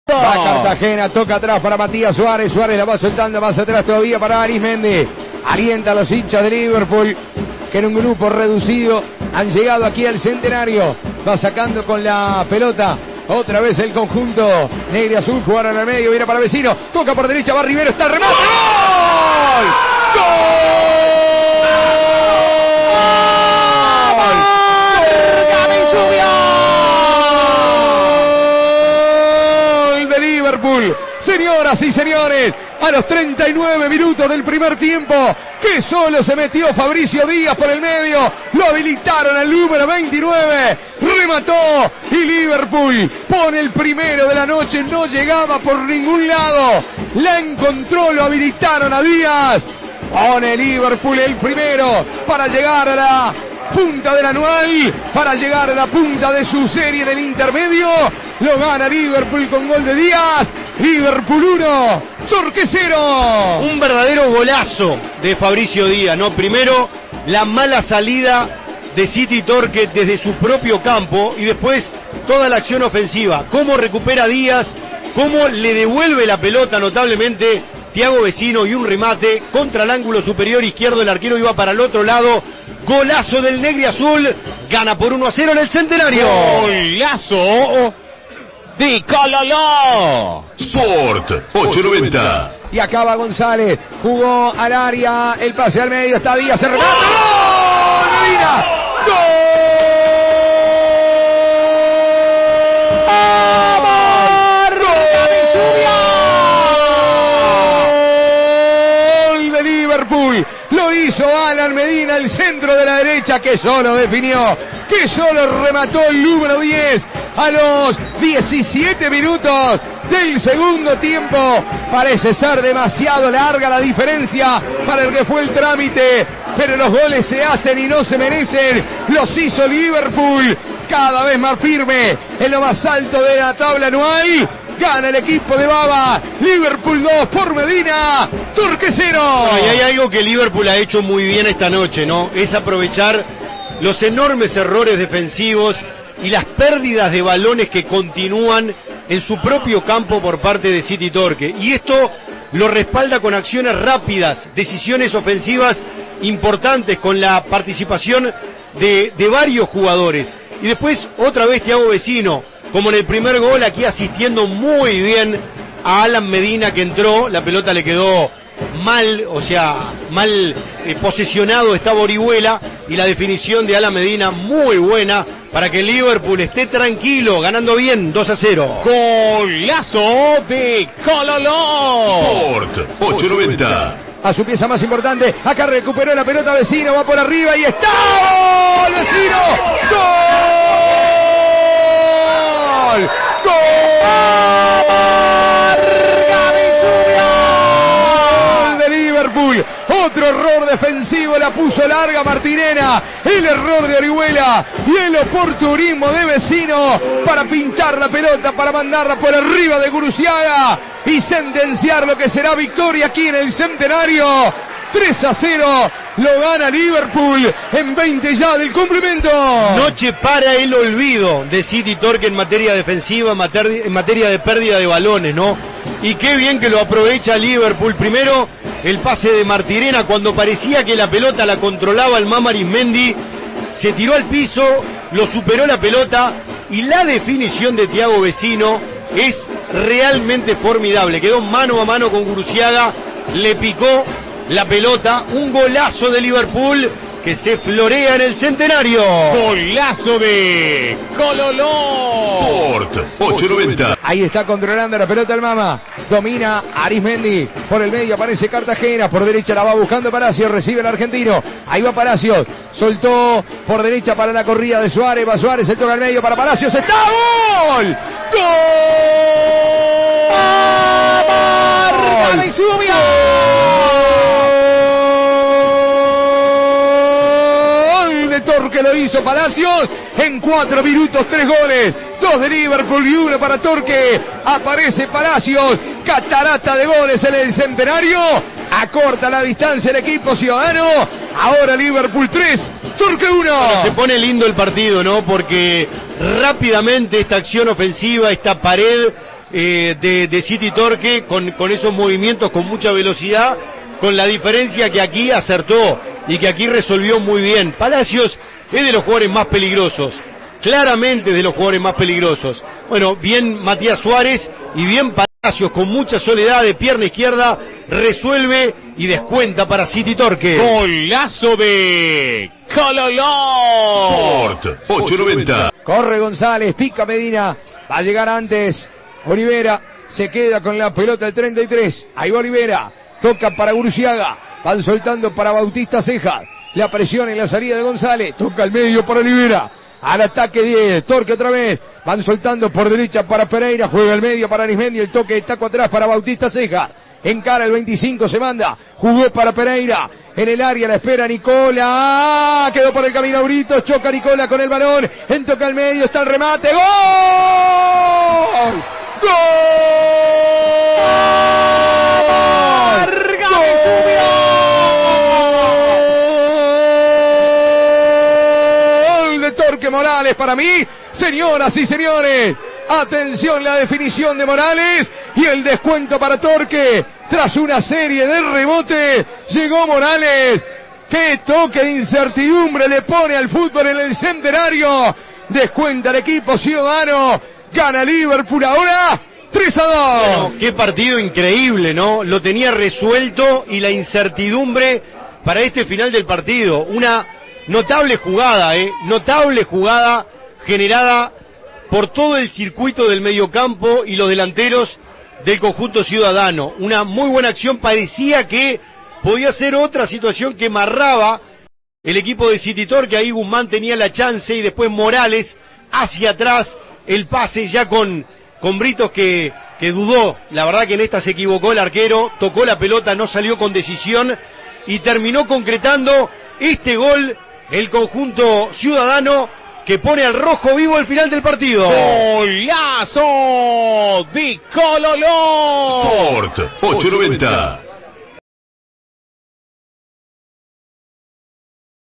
Torque 2 Liverpool 3 - Intermedio 2022 (por Diego Jokas)
Los goles del partido, por Diego Jokas y Mauro Más para Sport 890.